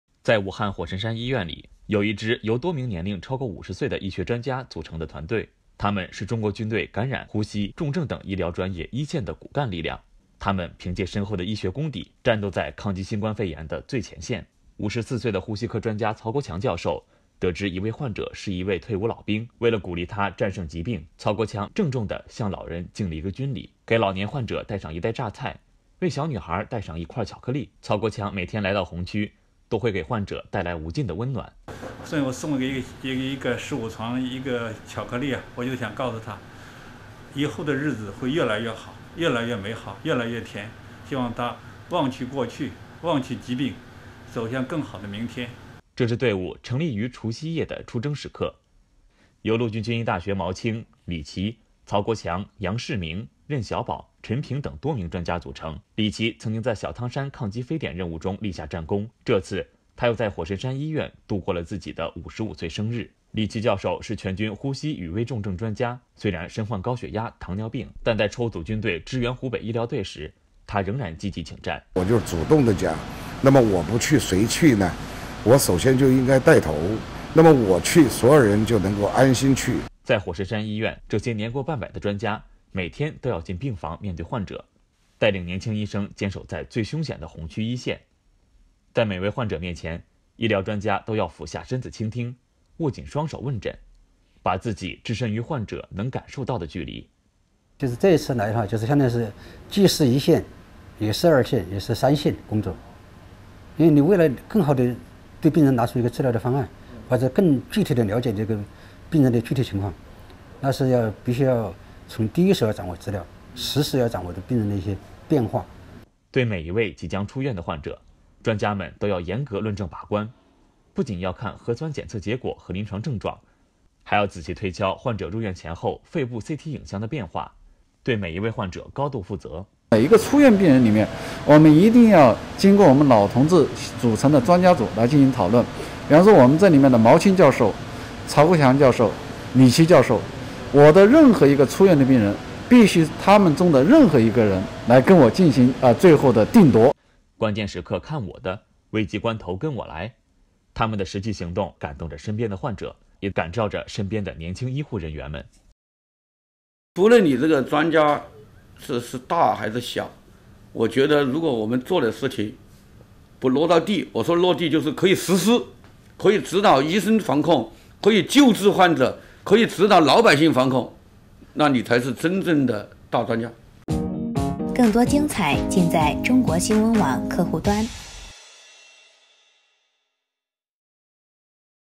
火神山医院的医学专家组：他们是这里的“定盘星”
在武汉火神山医院里，有一支由多名年龄超过50岁的医学专家组成的团队，他们是中国军队感染、呼吸、重症等医疗专业一线的骨干力量， 他们凭借深厚的医学功底，战斗在抗击新冠肺炎的最前线。